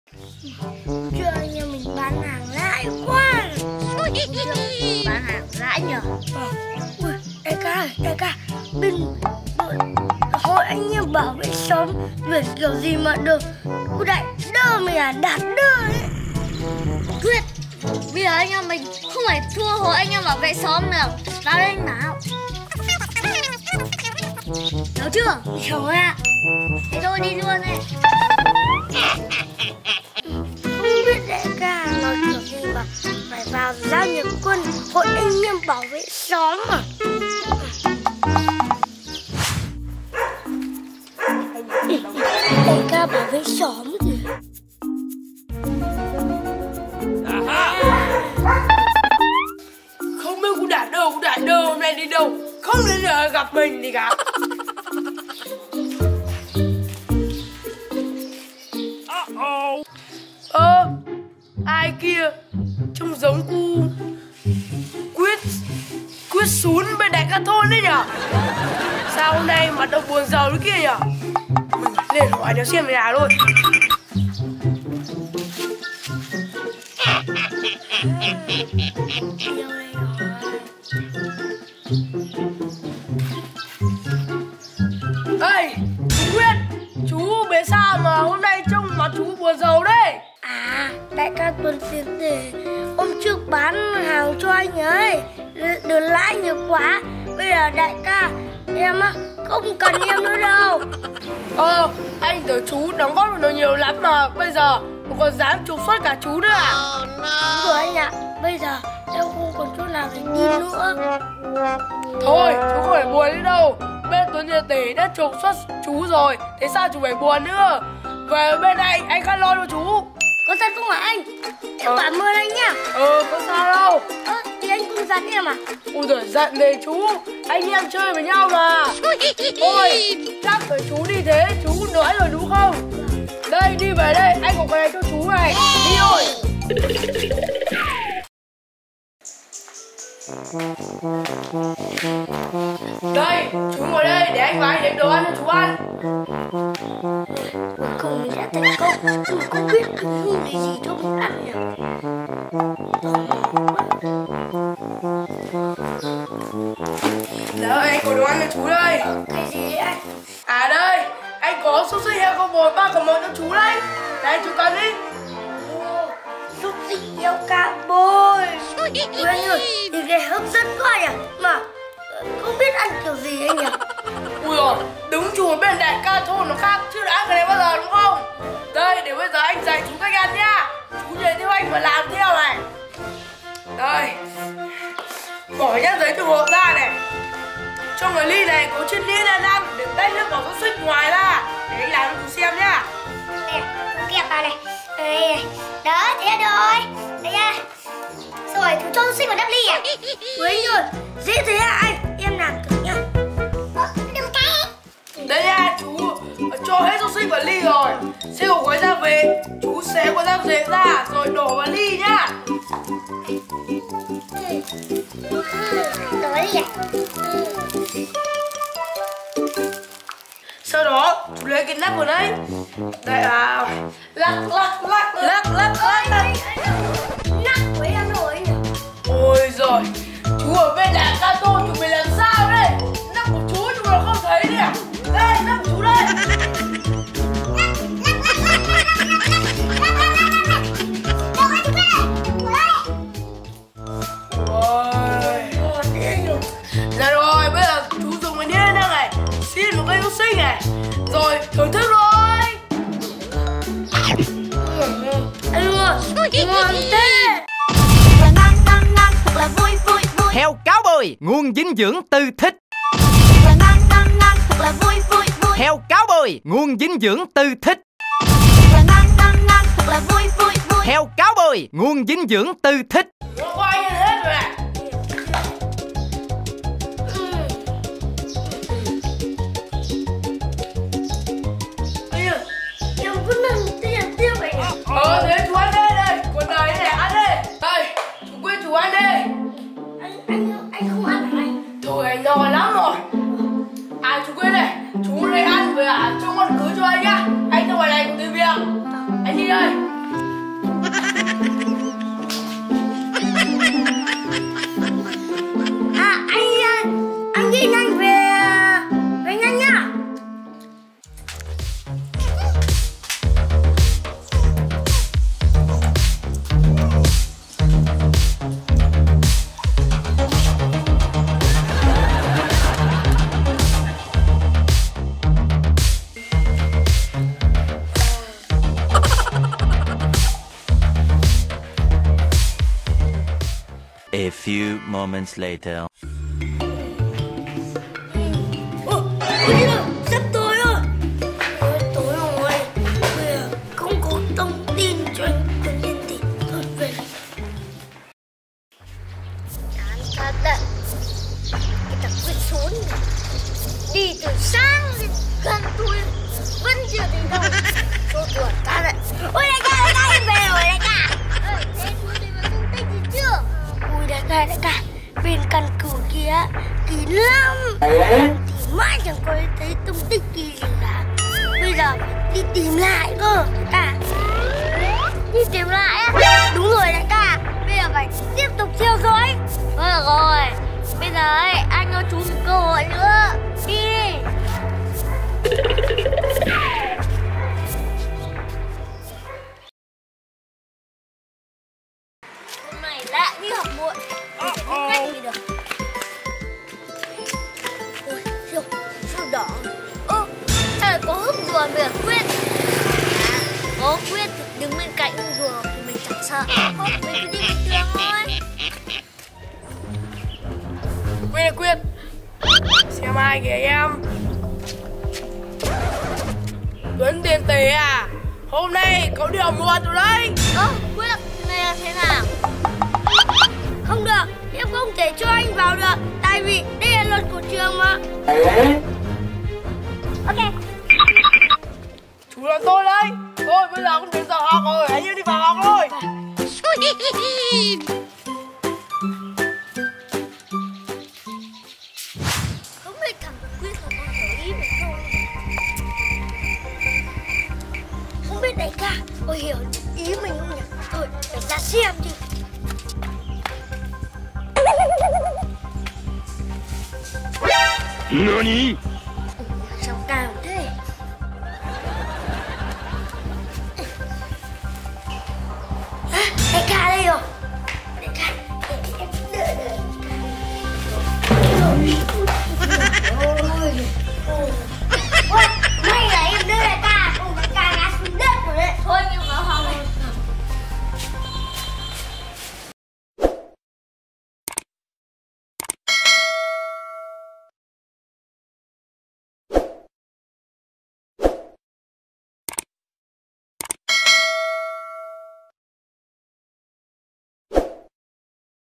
ĐẠI CA THÔN TẬP 4 ❤ Gián Điệp Hai Mang Phim Ngắn Hay ❤ Biệt Đội Lầy Lội
Phim Ngắn Coi Thường Hai Anh Em Nhặt Ve Chai Và Cái kết ❤️ Biệt Đội Lầy Lội

Hastag